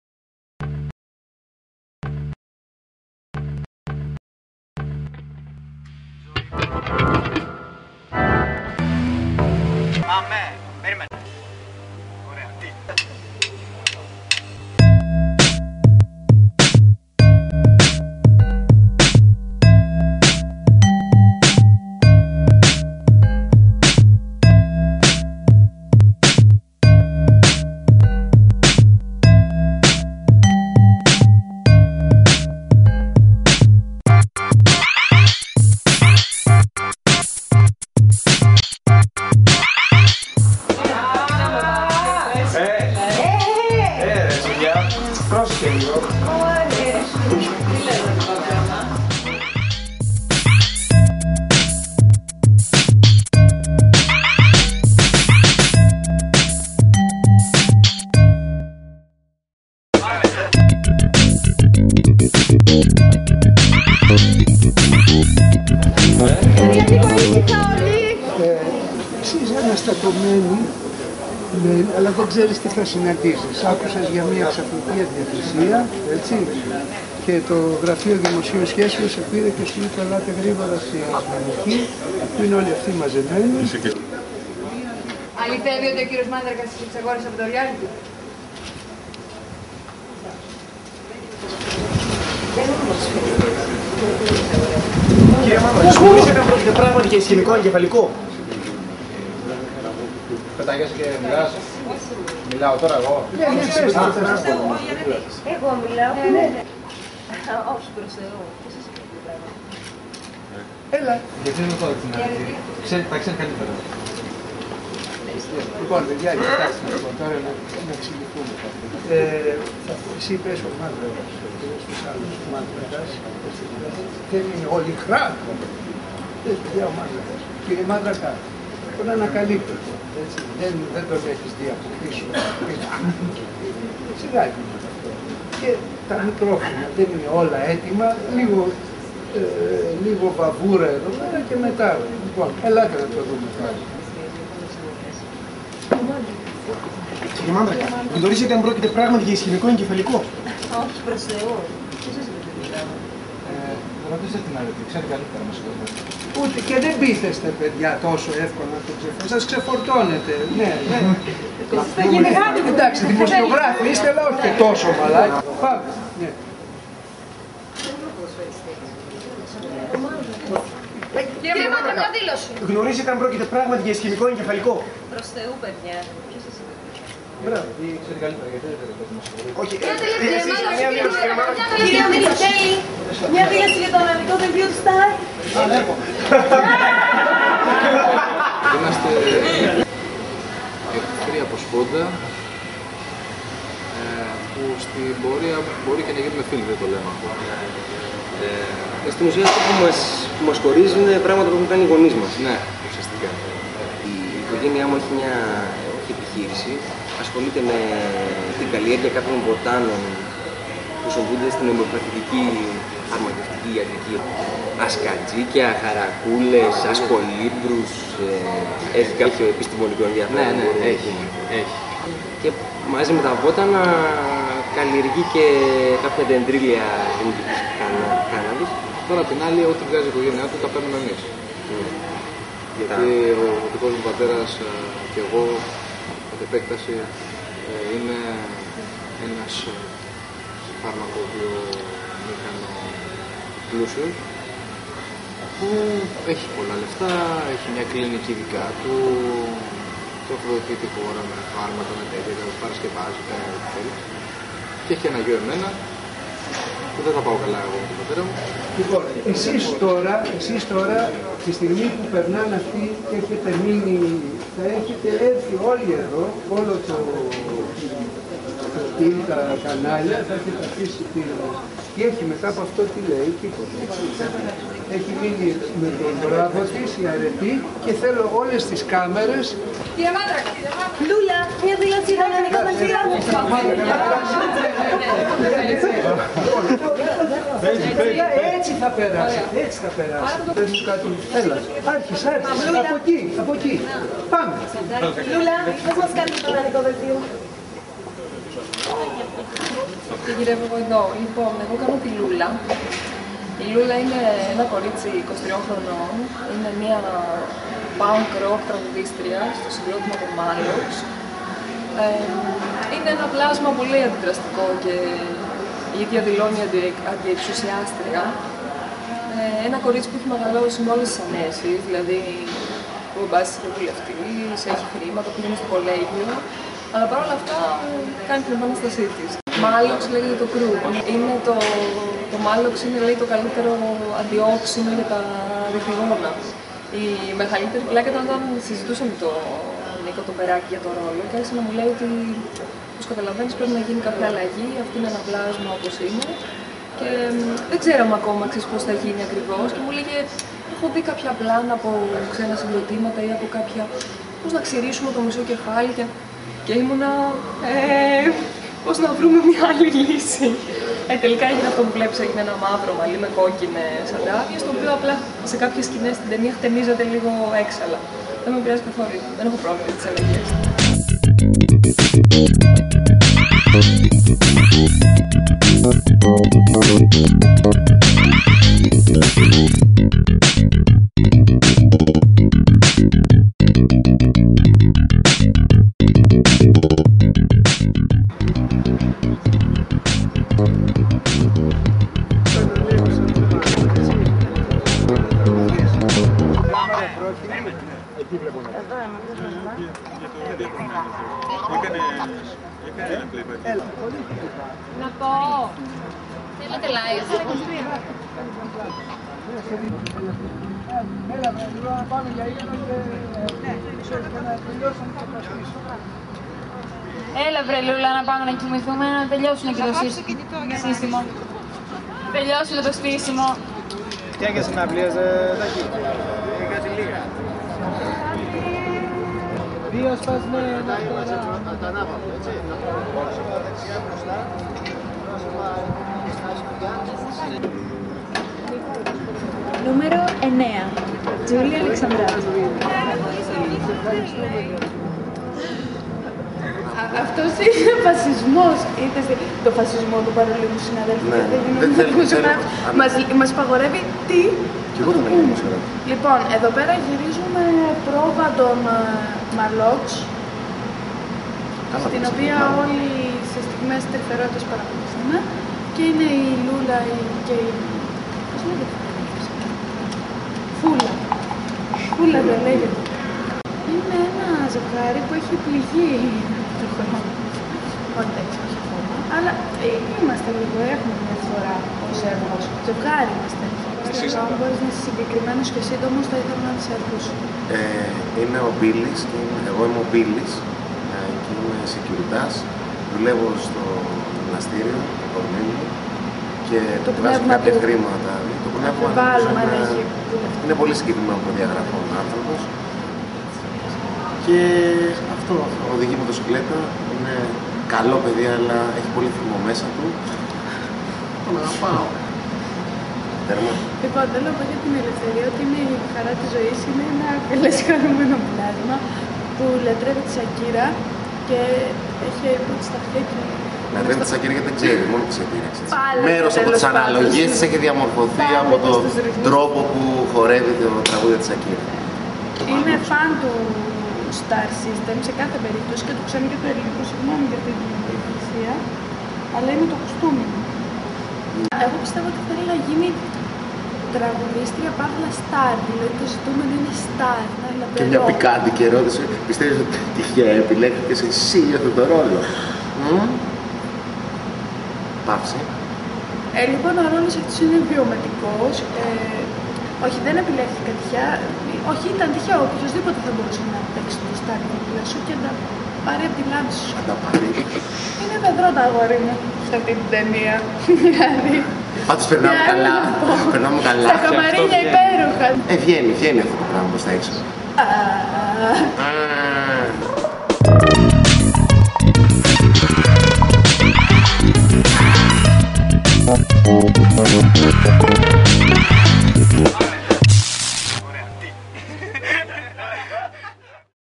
Psyxraimia: Set Visit
Movies for the Masses visits the set of Nicos Perakis' latest film, Psyxraimia.